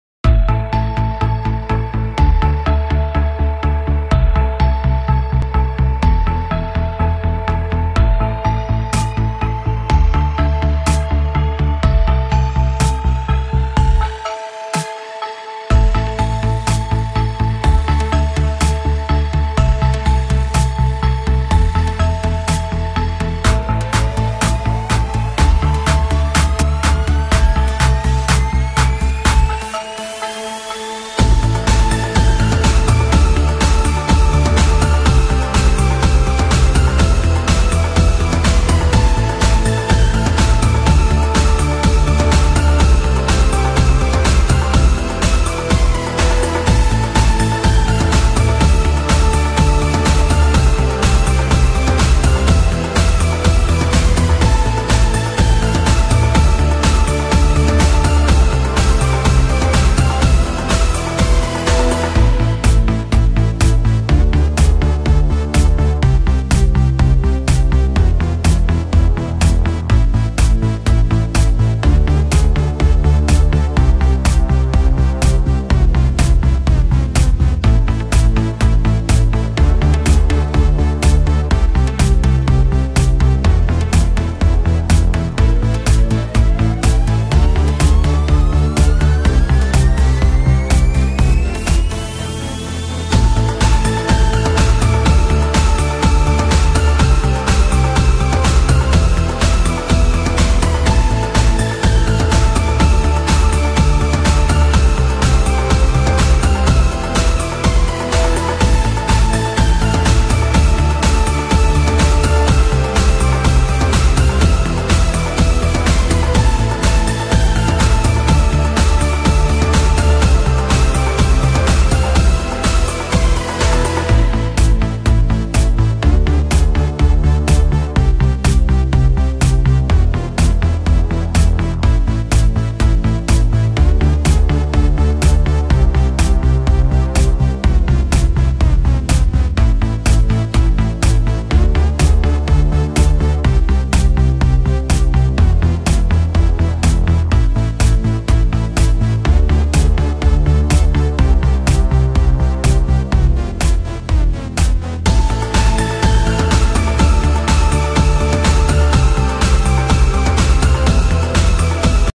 Honda Grom Custombike
Honda Grom modified in Thailand